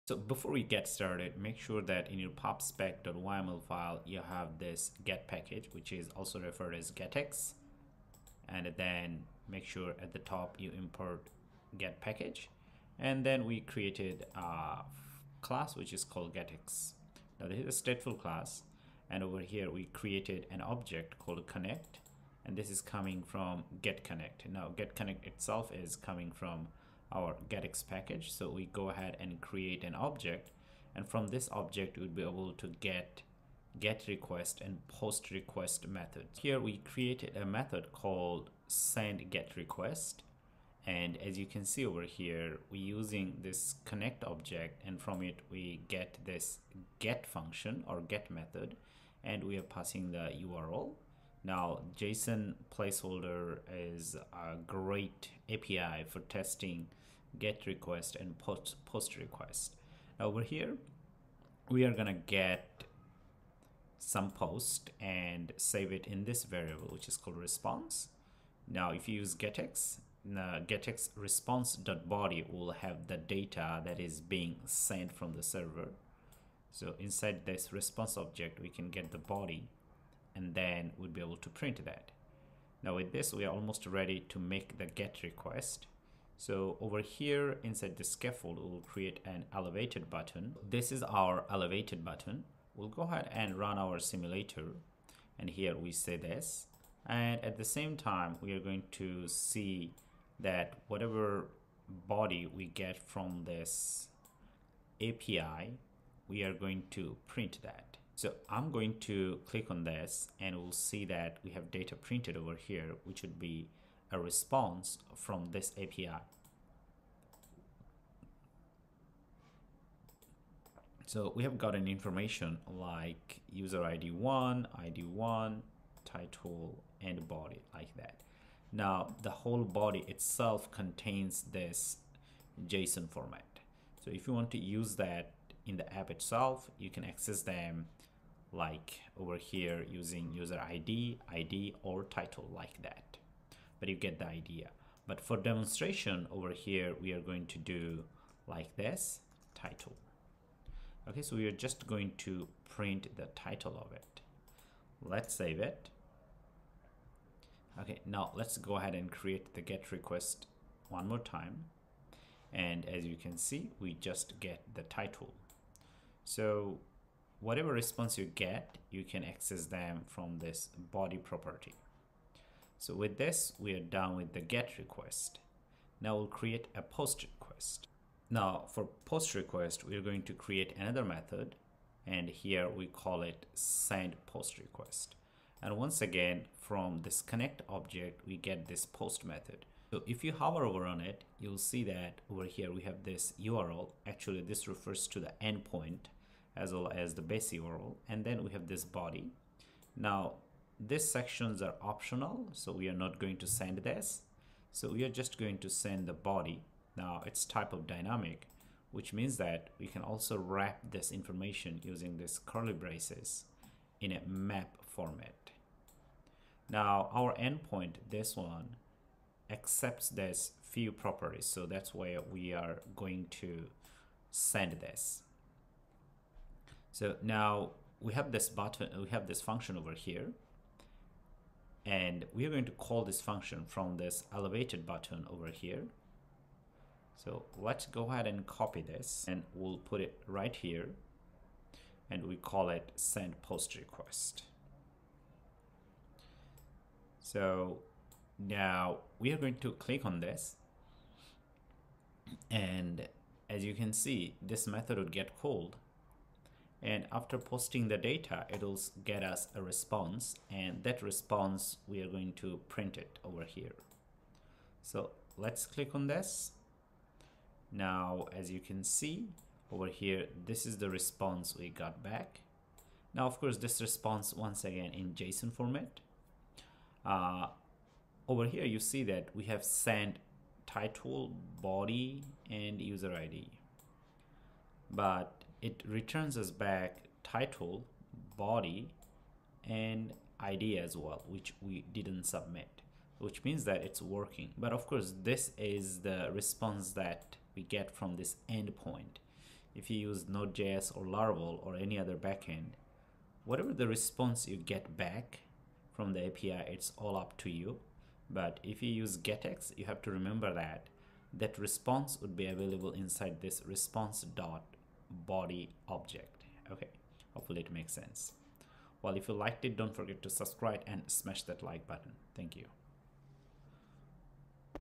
Flutter Getx Post/Get Request | Restful Api Http Request
Let's learn about getx post and get request. We will use getx getConnect to achieve this.